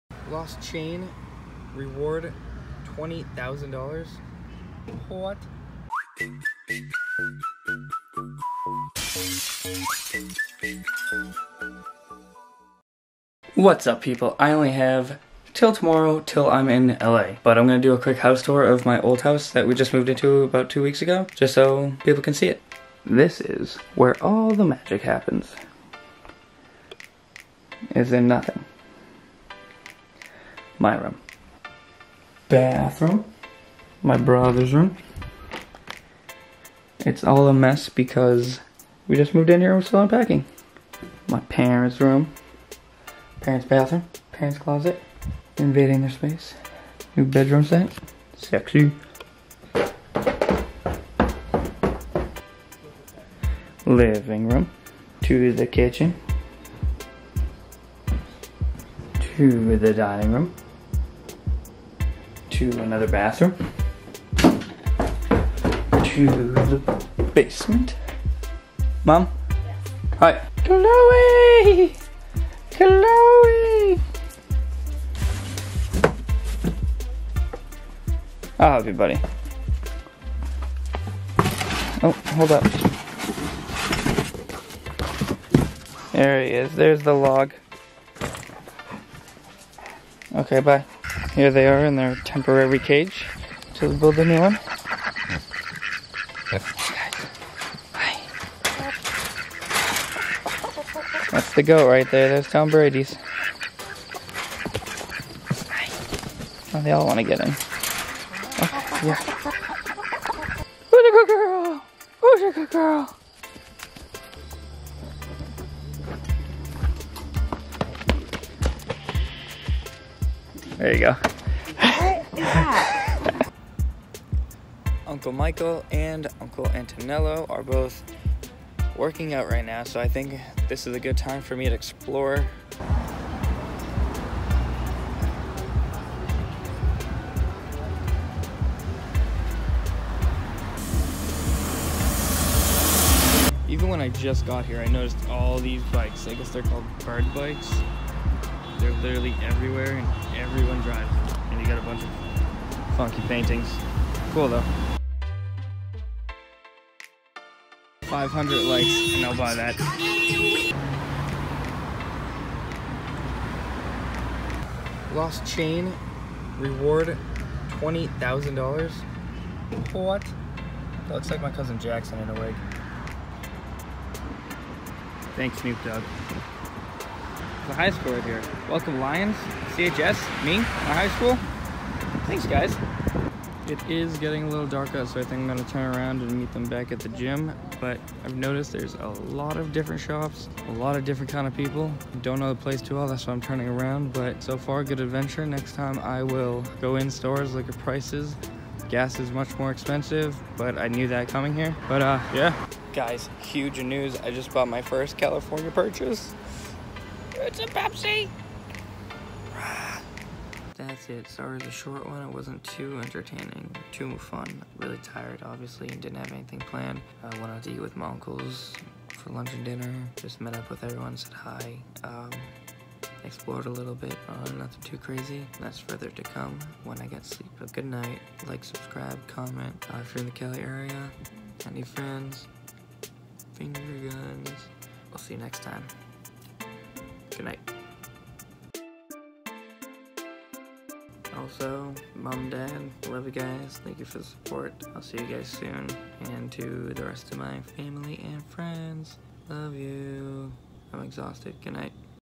MY FIRST DAY MOVING TO LOS ANGELES CALIFORNIA
VLOG#20

Today is a huge step hopefully in the right direction with my move to Los Angeles California!
Well here goes nothing!

Joshg543

Joshguarnera

Joshguarnera